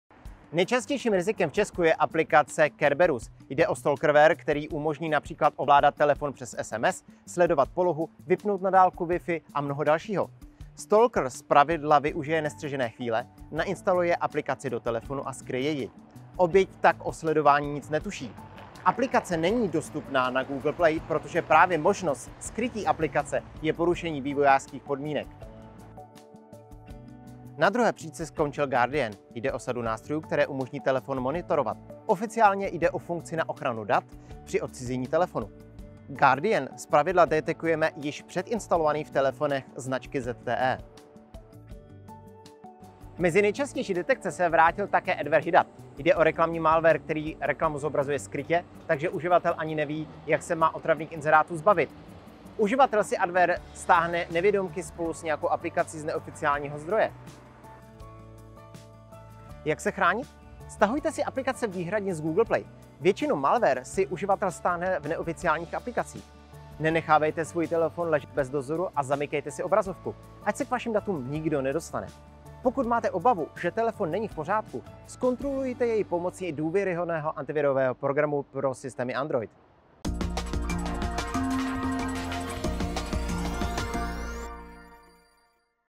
Aplikace z neoficiálních zdrojů ohrožují telefony s Androidem
Nejvýraznějším rizikem pro české uživatele je stalkerware a adware. V obou případech je pro uživatele infikovaného zařízení velmi obtížné se problémové aplikace zbavit. Malware obvykle lze stáhnout z neoficiálního zdroje, například webové stránky či fóra.